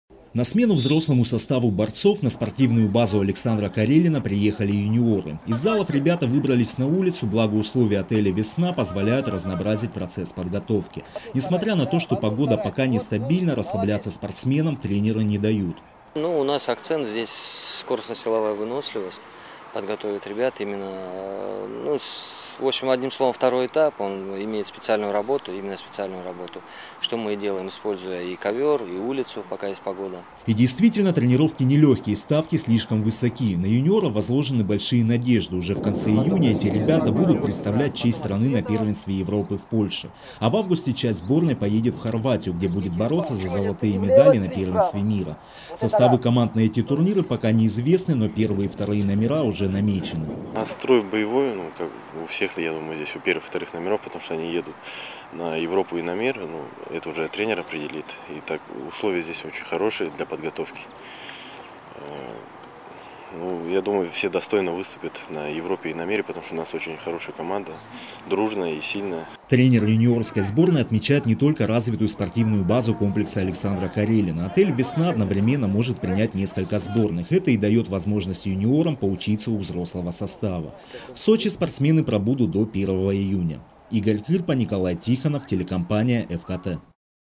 В Сочи проводит сборы команда России по греко-римской борьбе Новости 24 Эфкате Сочи
Отель «Весна» снова принимает спортсменов. На этот раз на подготовку перед серозными турнирами приехала юношеская сборная России по греко-римской борьбе. В этом году на спортивной базе Александра Карелина они проводят сбор первый раз.

Сочи. Новости.
Телеканал Эфкате.